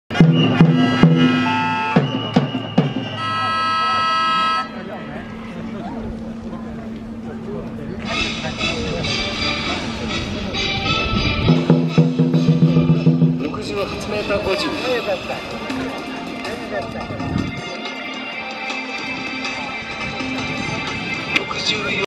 松橋亜希 選手 第98回全日本ノーマル １本目 68.5メーター
ドリームリンク所属の亜希選手。１本目は、女子組12位。
2019/10/26(土) 宮の森シャンツェ